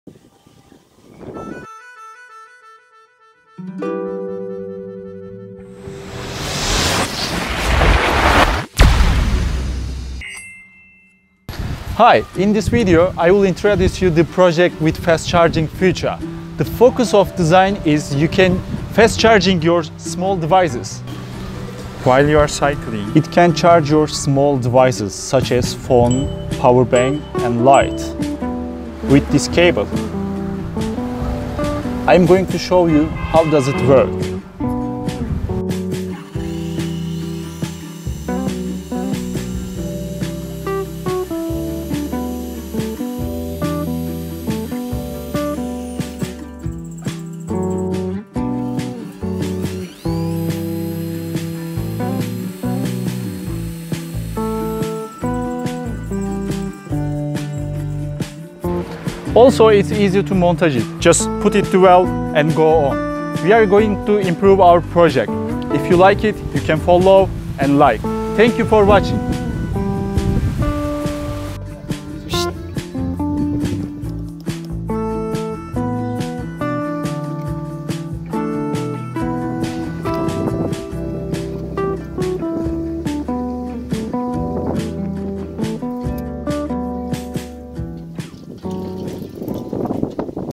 Free Energy Generator Fast Charging Small Device
Thank You my friend Hakan for helping me
PROJECT KEDIGOZ ADVANTAGE
While you are cycling, you able to charge your small devices.
It's really easy to assemble to the bike, you can attach to the rim of the bike, also it's portable you can take off whenever you want.
Current Electric Value (processing)
Vmax  ~ 8,87V
Amax  ~ 0,41A
Rpm    ~ 150-200